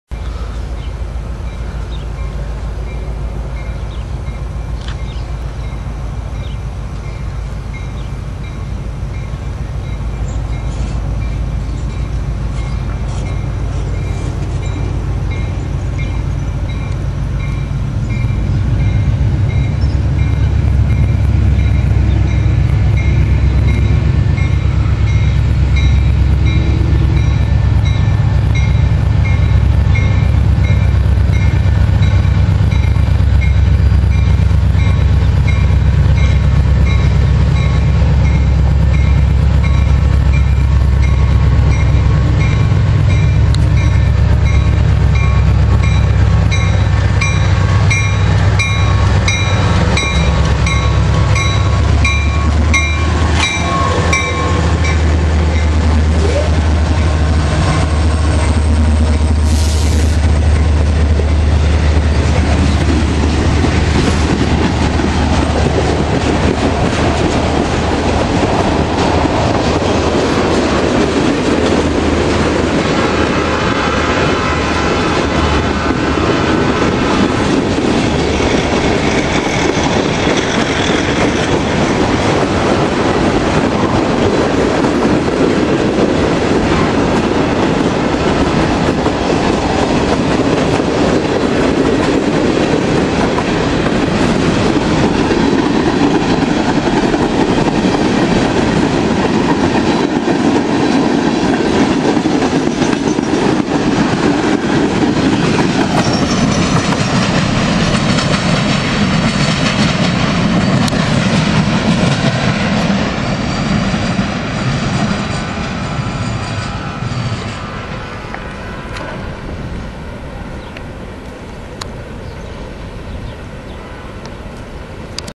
NS At allinace ohio
an NS train comes down to the amtrak station comes off the NS Cleveland line onto the Fort Wayne line at allinac ohio with the 8416 leading the train.